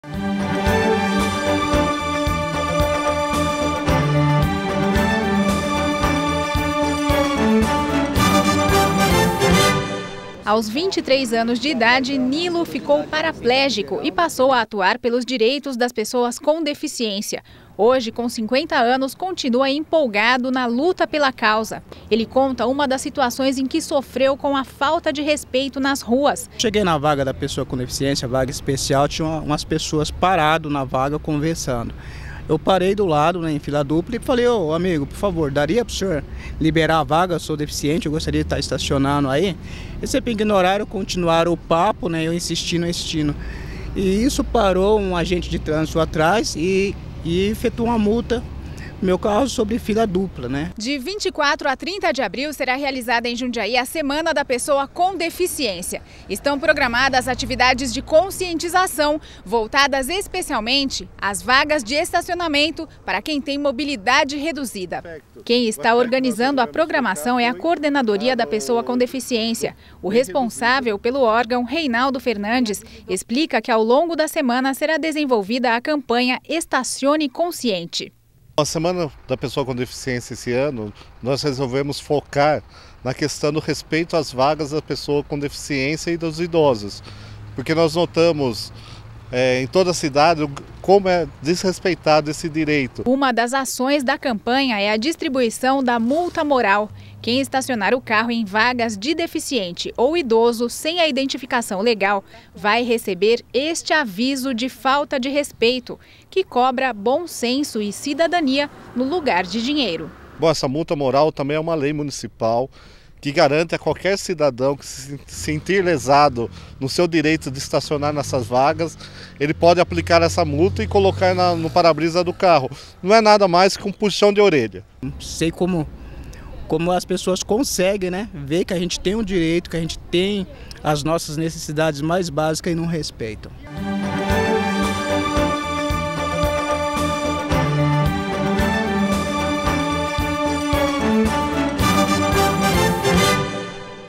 Campanha Estacione Consciente aplicará "multa moral".
A partir desta quarta-feira, dia 24, terá início a campanha Estacione Consciente. A ideia é mostrar aos motoristas a importância das vagas reservadas a deficientes e idosos. Quem desrespeitar poderá receber uma "multa moral".
::Matéria produzida pela TVE Jundiaí::